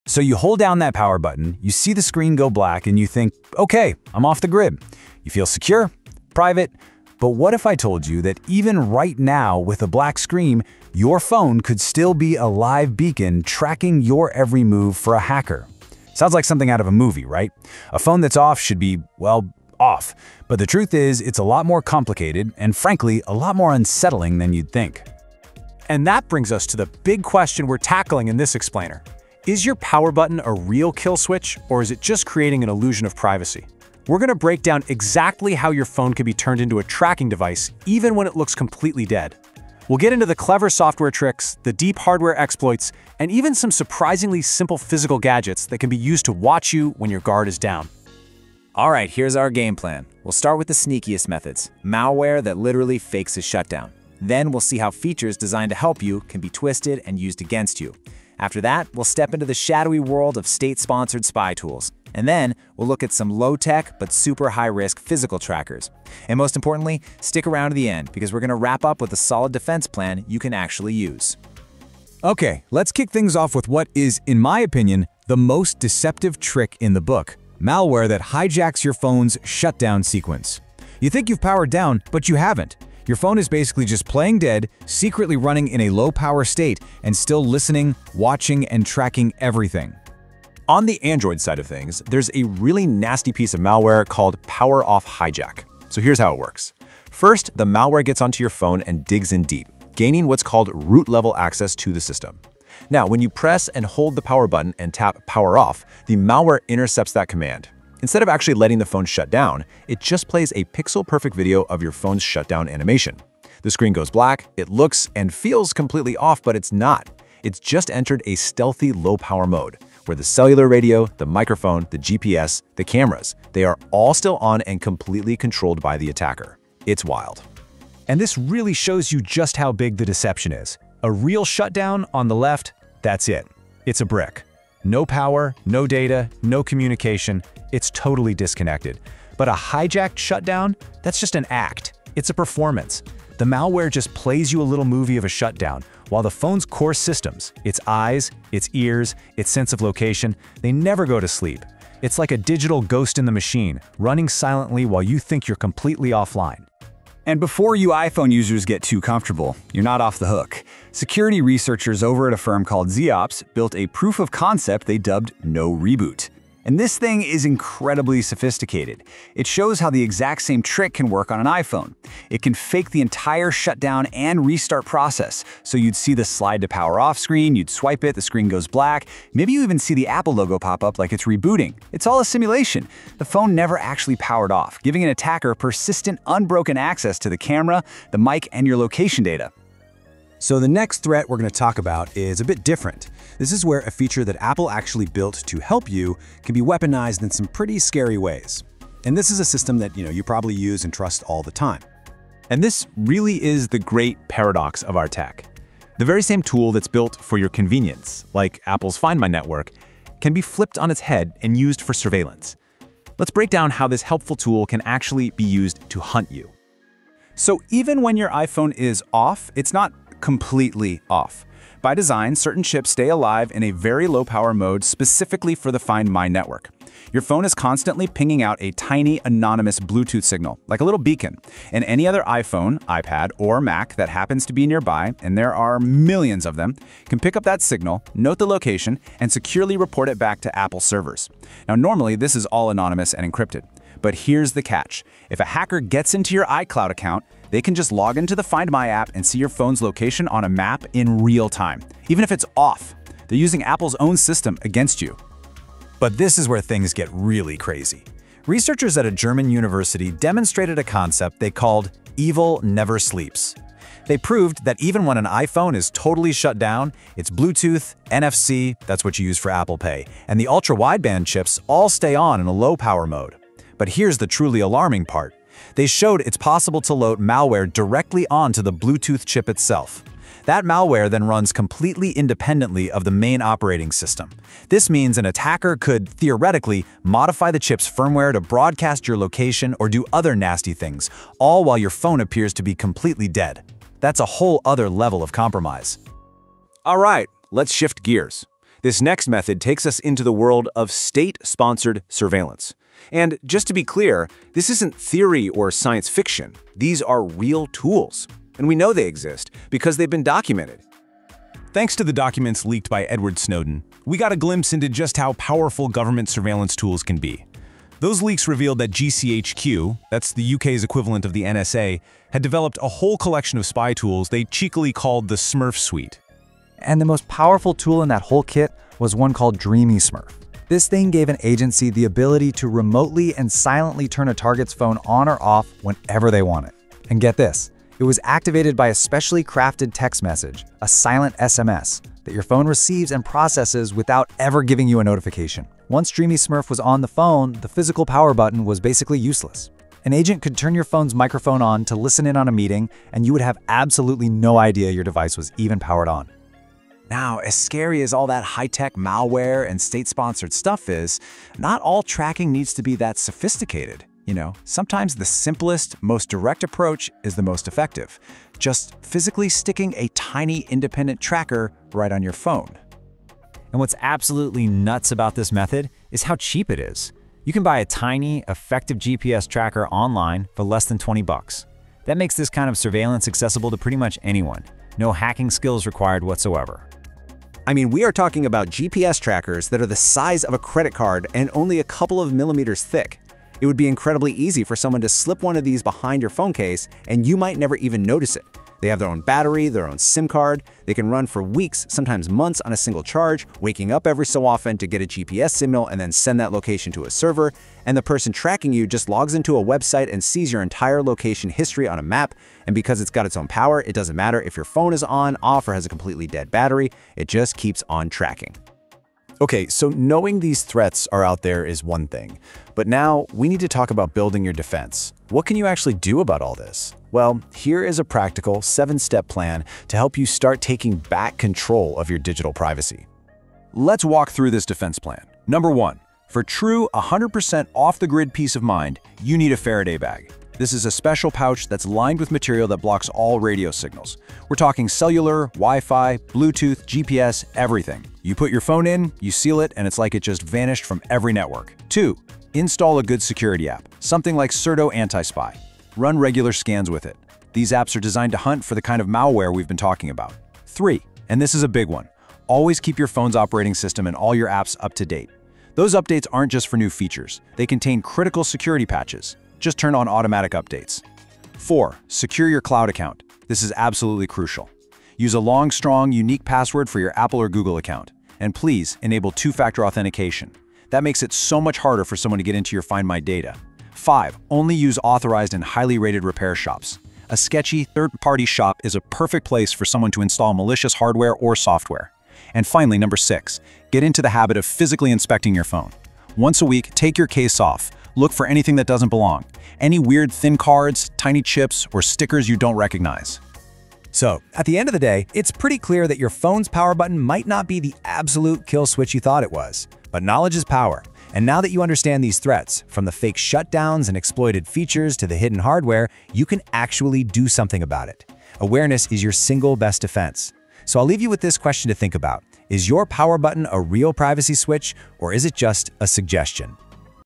How Hackers can track your phone even when it's powered off
This video reveals the shocking truth — hackers and government agencies can track smartphones that appear completely shut down.
You’ll learn:
🔹 How malware can hijack the shutdown process on both Android and iPhone
🔹 How always-on chips used for features like Find My can be exploited
🔹 How state-sponsored tools like the NSA’s Dreamy Smurf can track devices remotely
🔹 How attackers can use tiny physical GPS trackers hidden in your phone case
To protect yourself, the video also covers practical security tips:
✔️ Use a Faraday bag to block all wireless signals
✔️ Install a trusted mobile security app
✔️ Enable two-factor authentication (2FA) on your Apple or Google account
Stay one step ahead of hackers and surveillance tools.
🎥 Watch until the end to learn how to truly make your phone untrackable.
This video presents technical information for educational and cybersecurity awareness purposes.
No actionable instructions are provided to attack real systems.
The goal is to understand risks in order to improve defense, detection and digital hygiene.